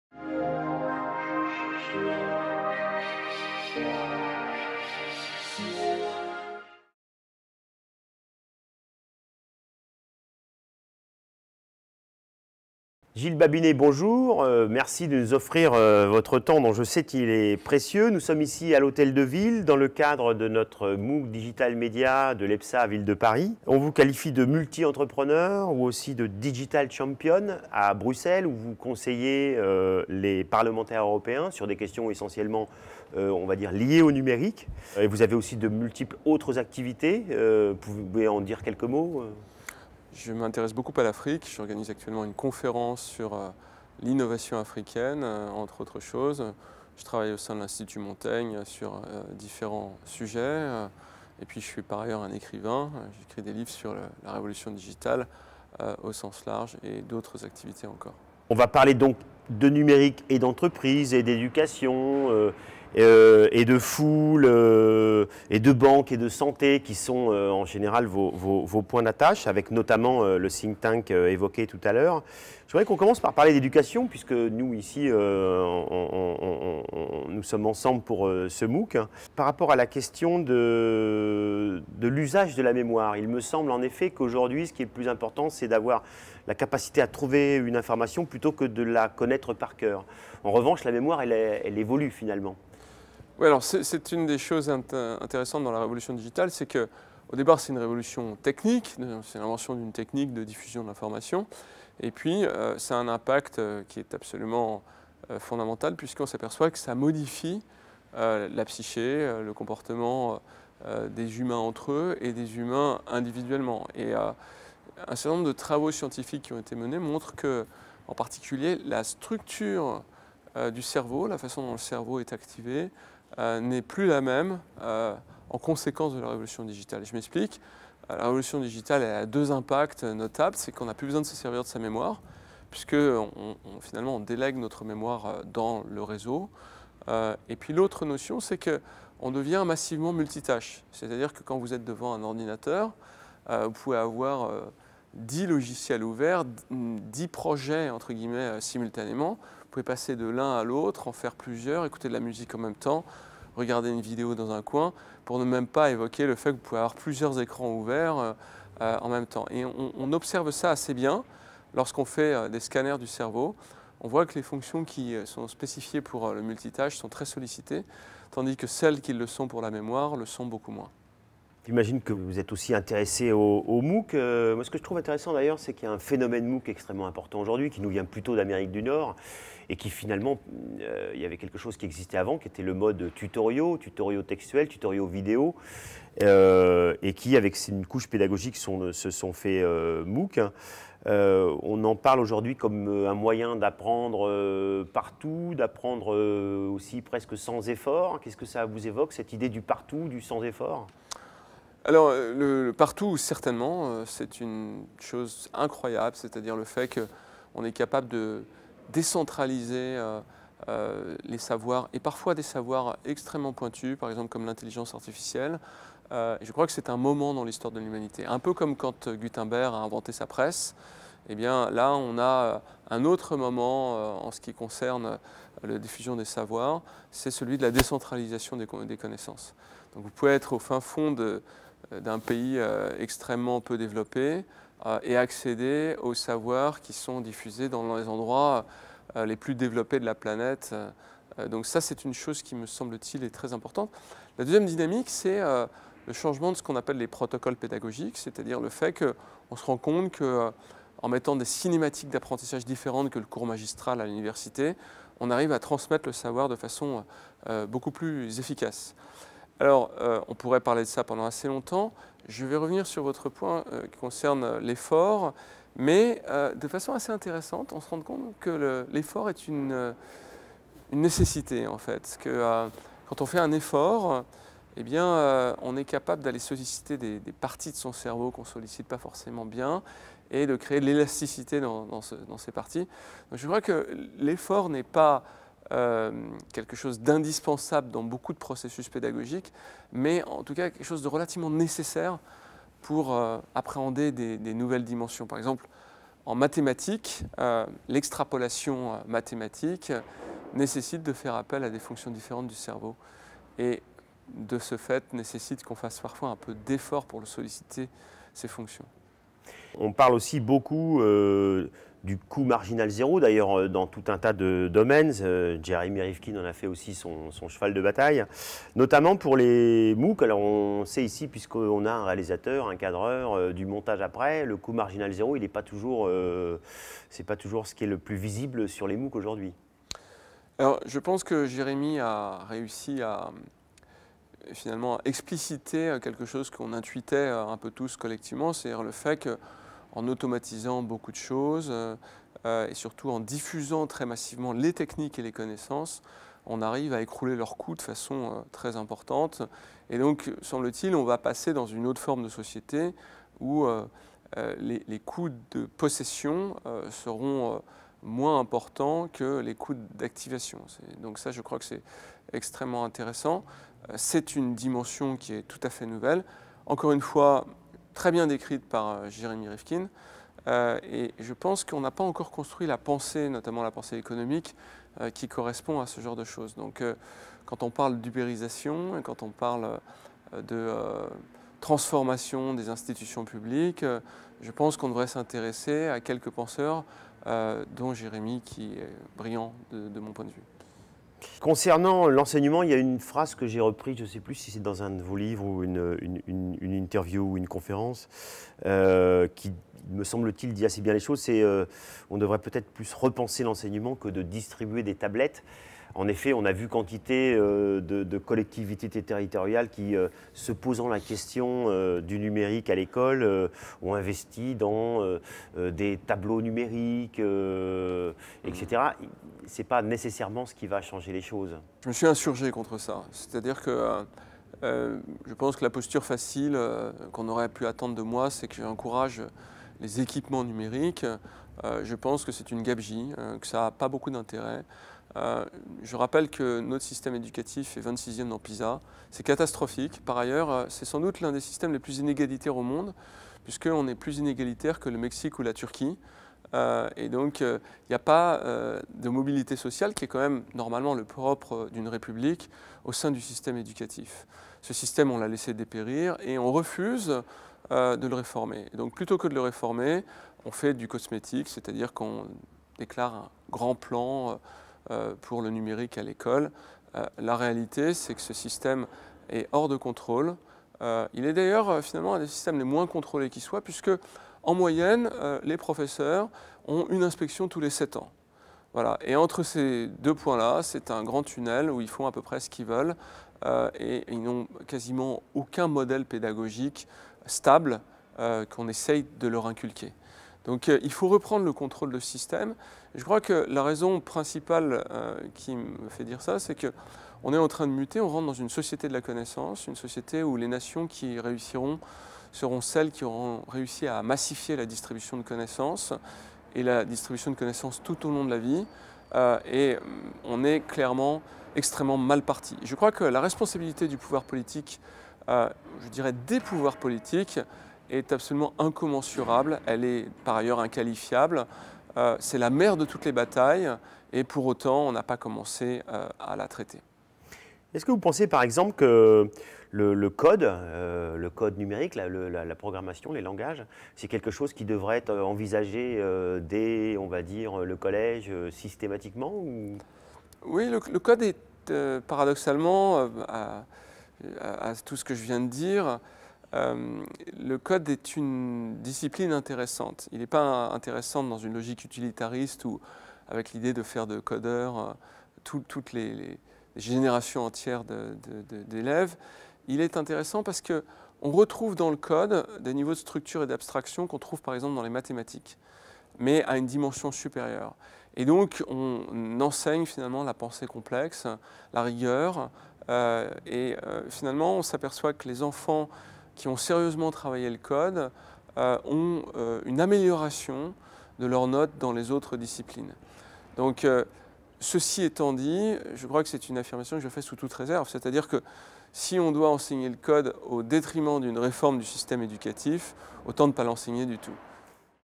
Gilles Babinet - 1. Du numérique aux data
Gilles Babinet a fondé de nombreuses sociétés dans des domaines aussi divers que le conseil, le bâtiment, la musique mobile, les outils décisionnels… Il a été président du Conseil National du Numérique.
Un cours réalisé pour le MOOC Digital Media de l’École professionnelle supérieure d'arts graphiques de la Ville de Paris (EPSAA) animé par Dominique Moulon à l’Hôtel de Ville de Paris et coproduit par MCD.